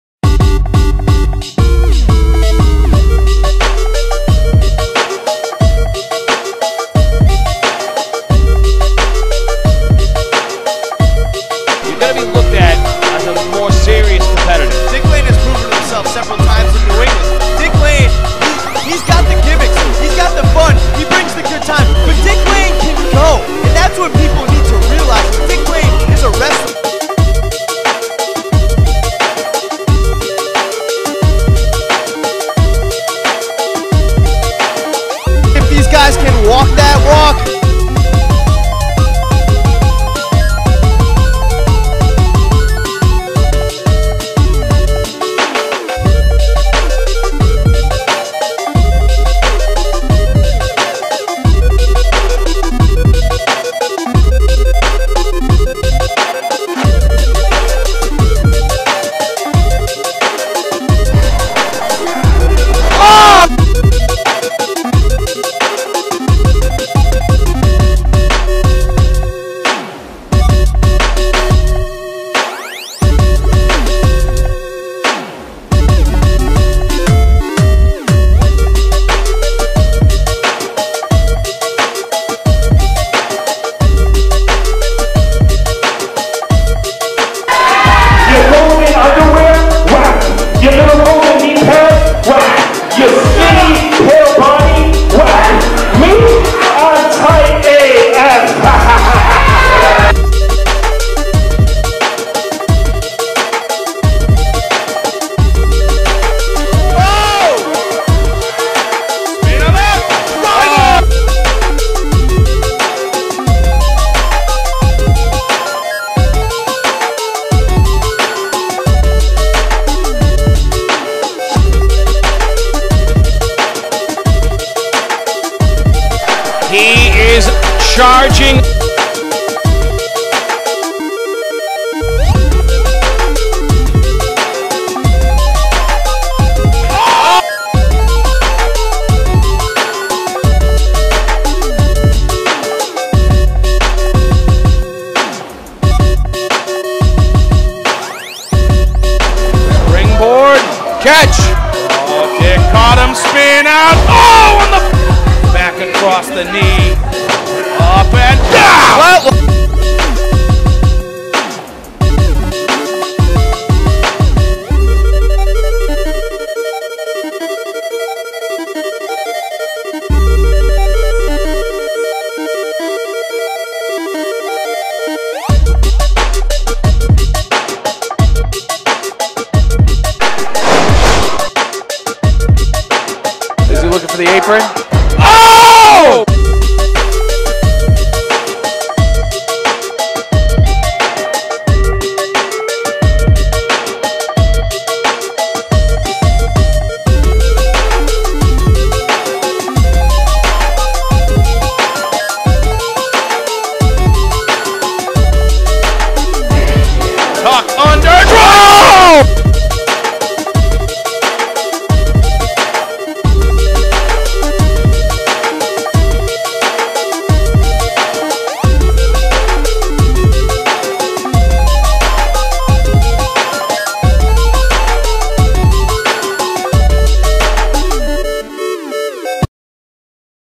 Dick Lane RWA/PG 2019 Compilation
Dick Lane doing wrestling moves in the year 2019 from the RWA and PG promotions.